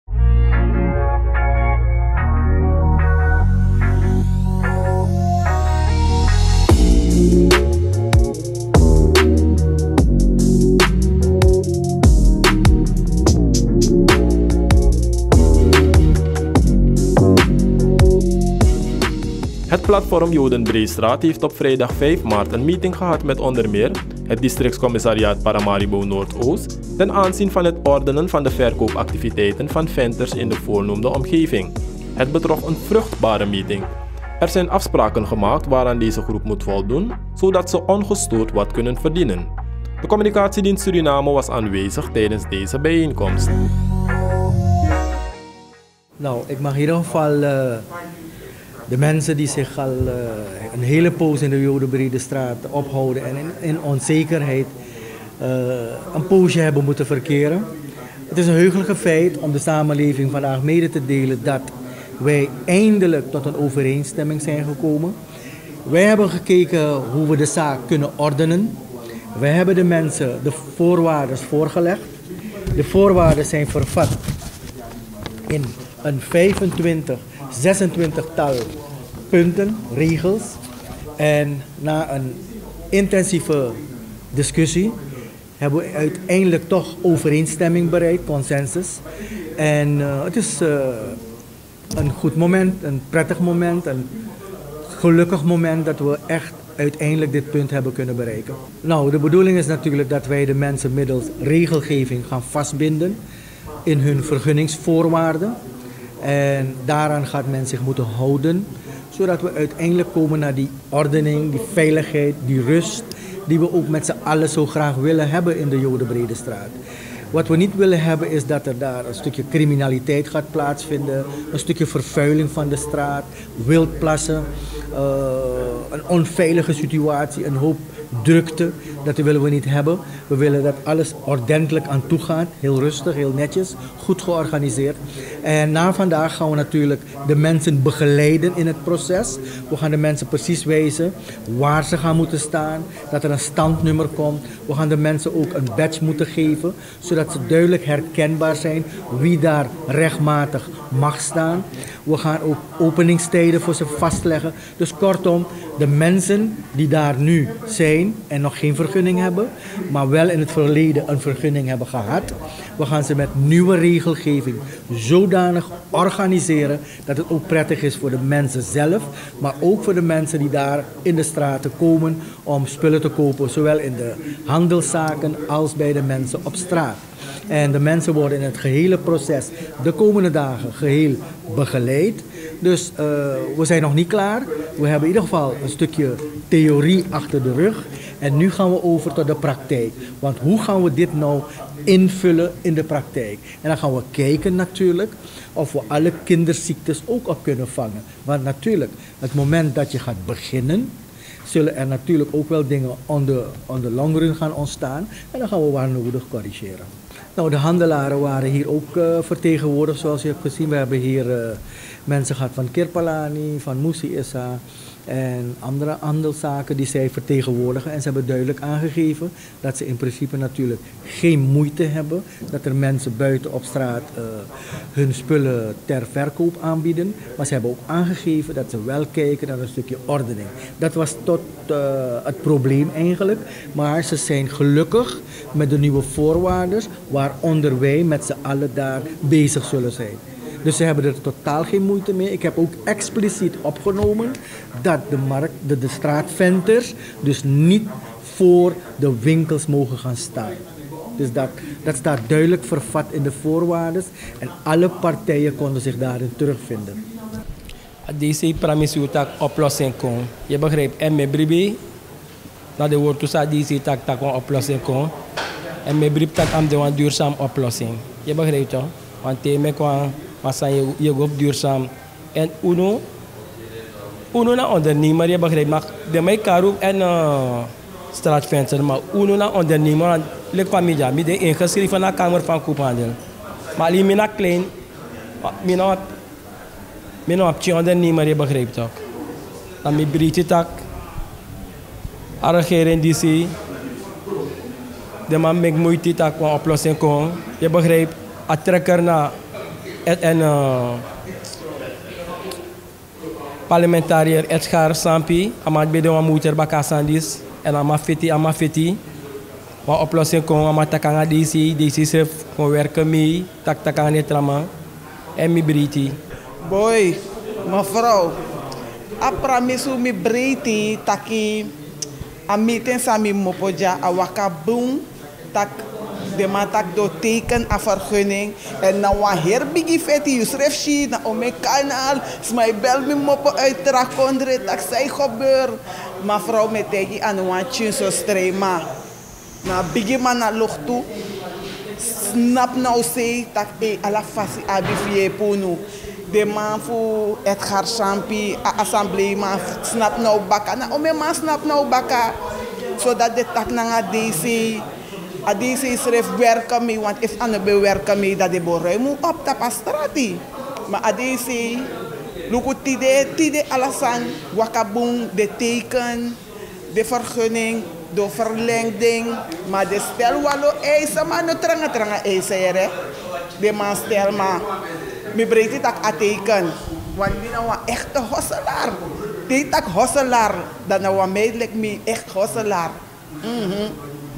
MEETING PLATFORM JODENBREESTRAAT
Het Platform Jodenbreestraat heeft op vrijdag 5 maart een meeting gehad met onder meer het districtscommissariaat Paramaribo Noord Oost ten aanzien van het ordenen van de verkoopactiviteiten van venters in de voornoemde omgeving. Het betrof een vruchtbare meeting. Er zijn afspraken gemaakt waaraan deze groep moet voldoen zodat ze ongestoord wat kunnen verdienen. De Communicatie Dienst Suriname was aanwezig tijdens deze bijeenkomst.